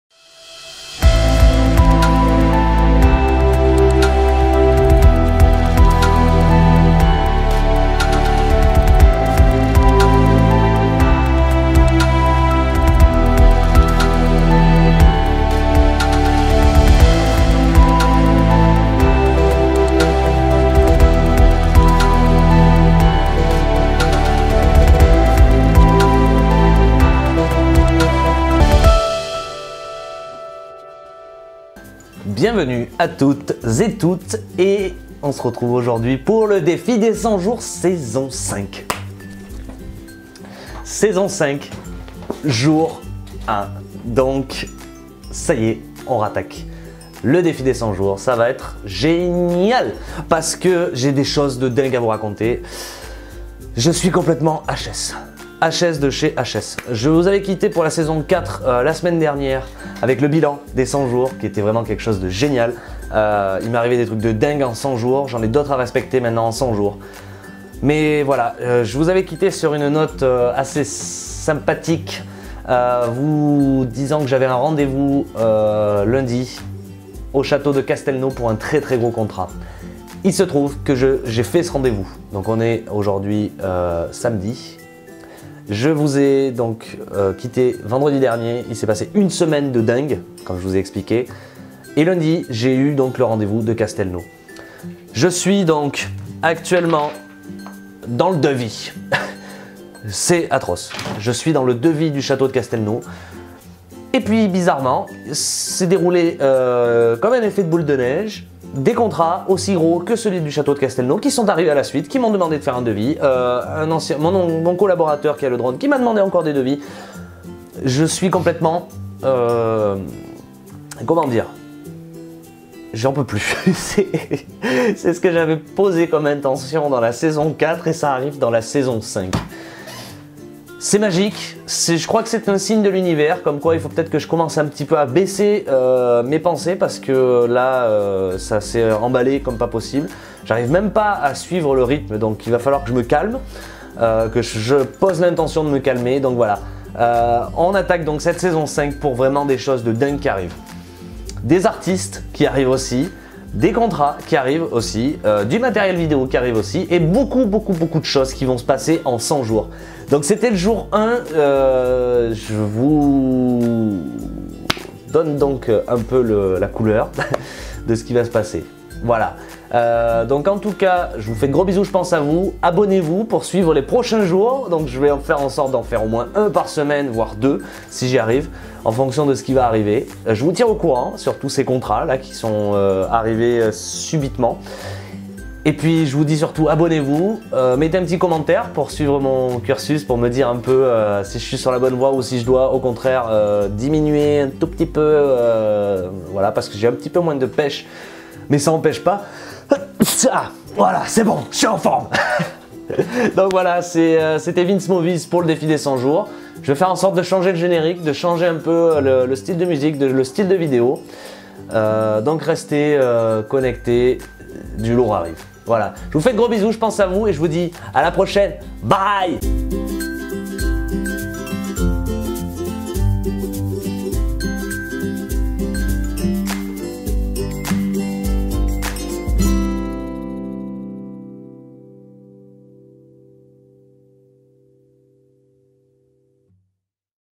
DÉFI DES 100 JOURS / SAISON 5 - JOUR 1 - On recommence !!
Salut tous le monde
Ca y'est la saison 4 est terminé et on attaque la saison 5
On recommence donc une nouvelle saison en posant les intentions !!

Le but ? Respecter au mieux ces nouvelles intentions !!
LET's Go !!!

Merci l'Univers !